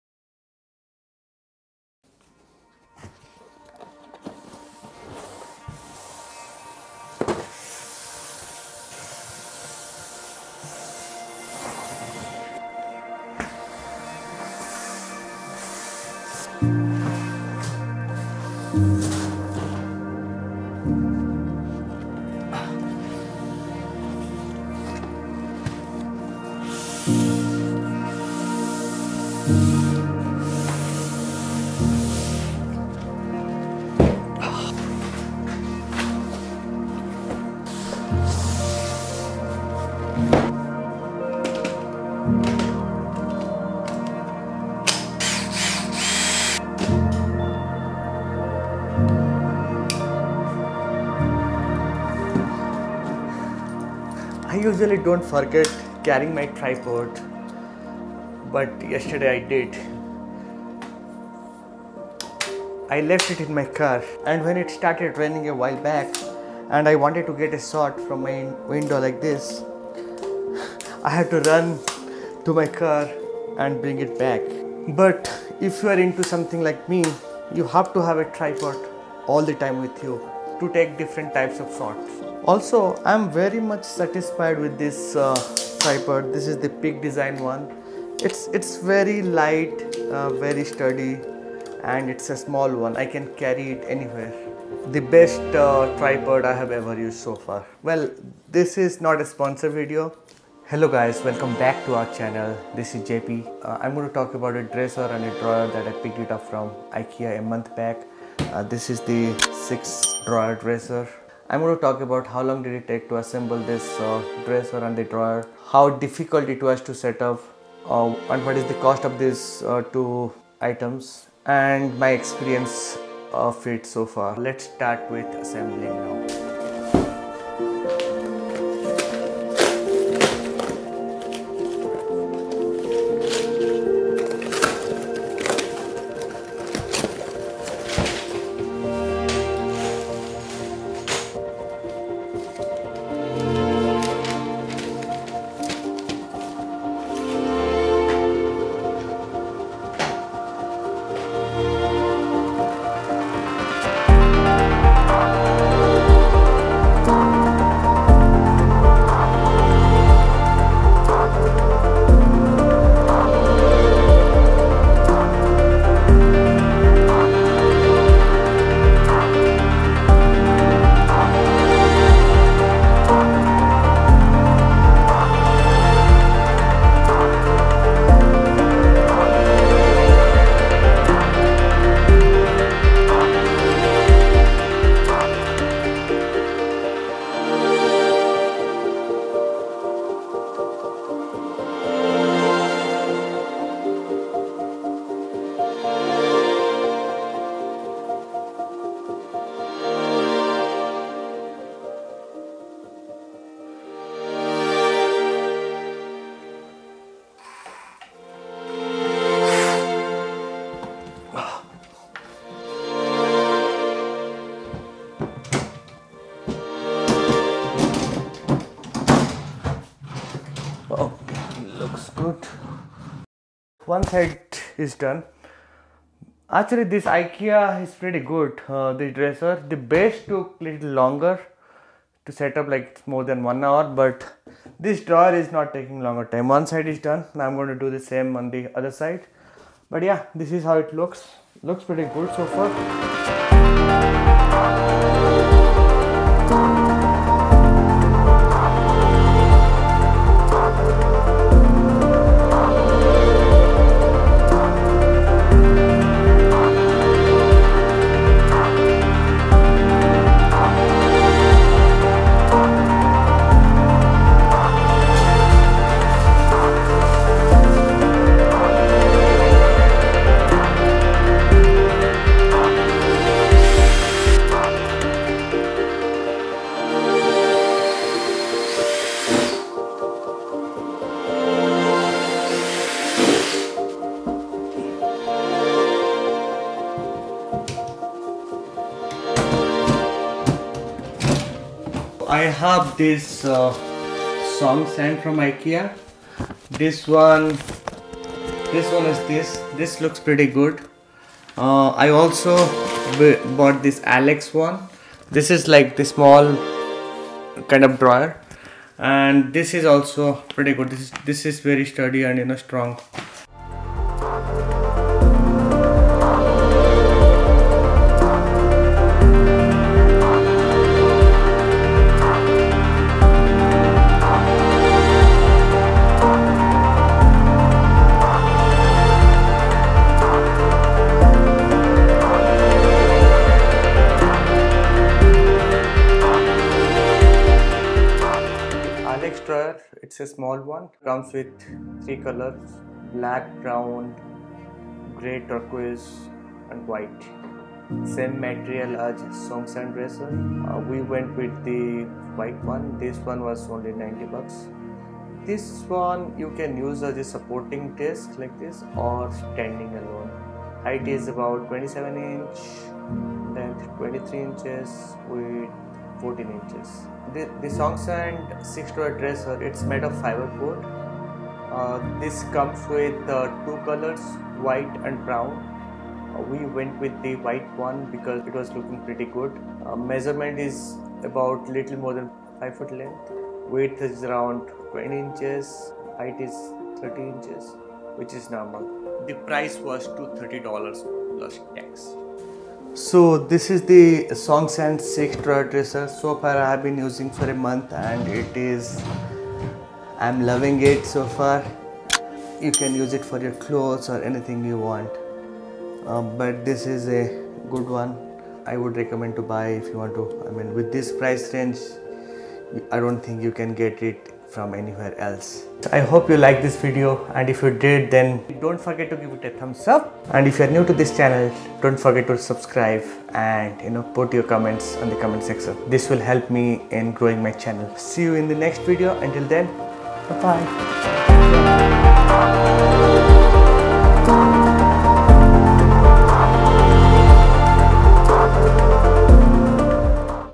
Ikea Dresser | Cinematic Video
SONGESAND 6 Drawer Dresser, ALEX drawer Unit by Ikea

SONGESAND 6 Drawer Dresser - $230
Assembling time - 2.5hrs
ALEX drawer Unit  - $90
Assembling time - 1.5hrs

Music I use from, Use below Link to get 30 days free music